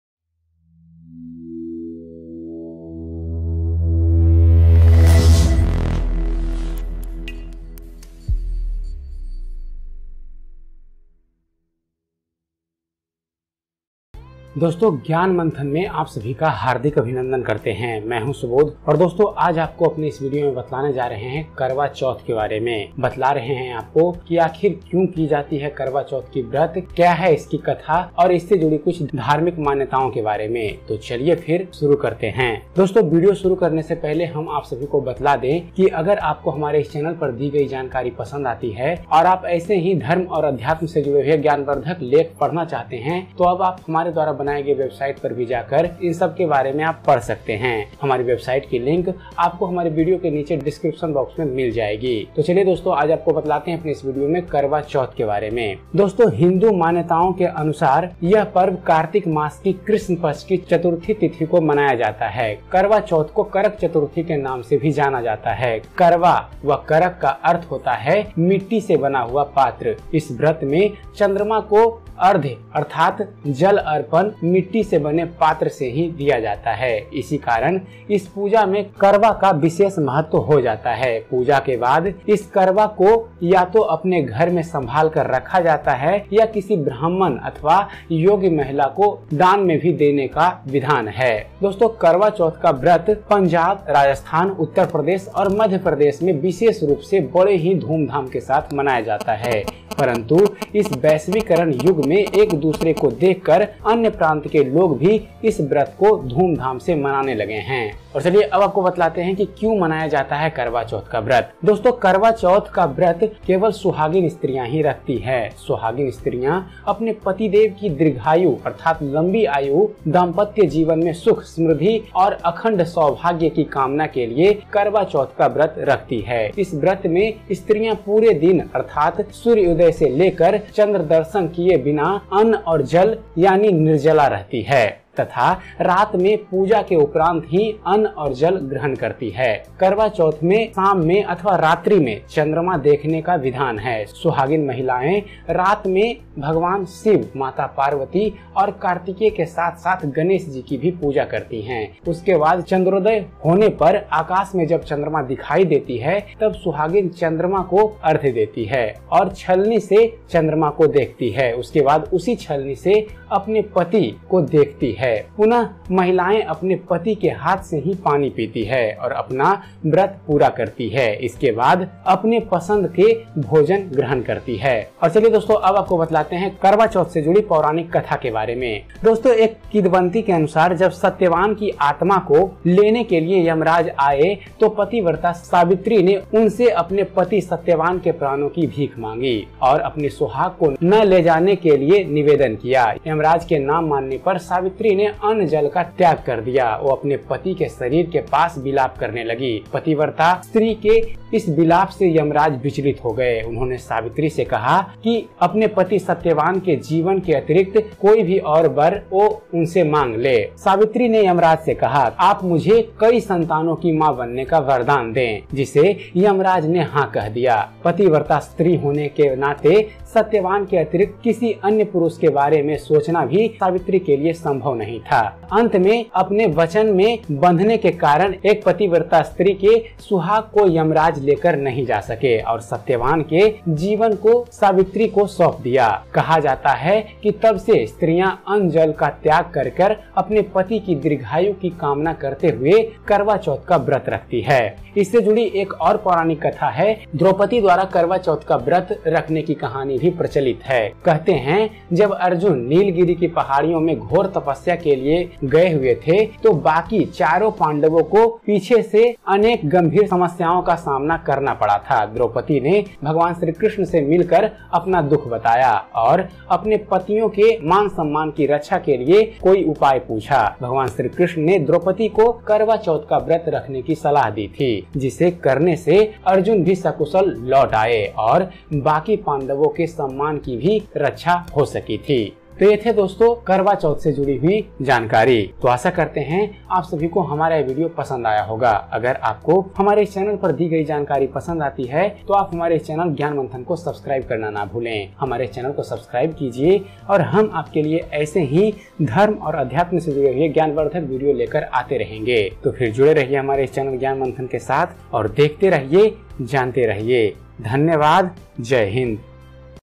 आखिर क्यों मनाया जाता है करवाचौथ? Importance Of Karwa Chauth (HINDI)
आखिर क्यों मनाया जाता है करवाचौथ? Karwa Chauth Pooja Vrat Vidhi  or Story in Hindi.